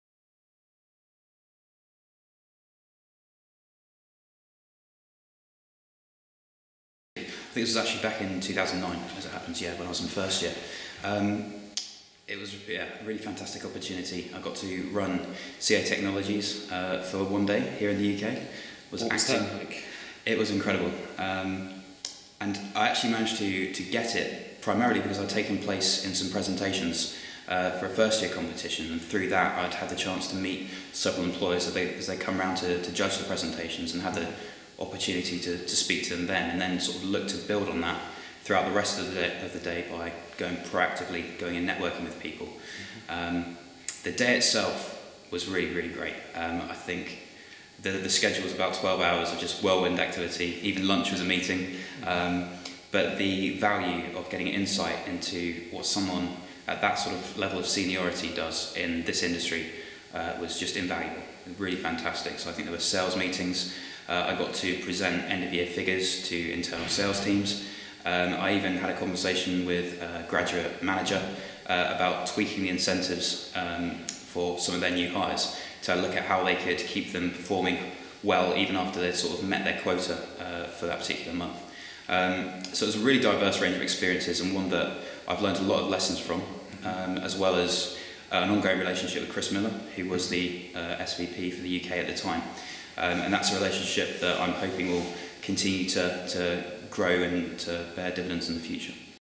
What was it like being CEO for a day at CA Technologies?
During his time on the ITMB degree, Andrew won the exclusive chance to shadow the CEO of CA Technologies for a day. This is a prize only given to ITMB students at the South ITMB Student/Employer event. Andrew gives us an insight on what the day entailed.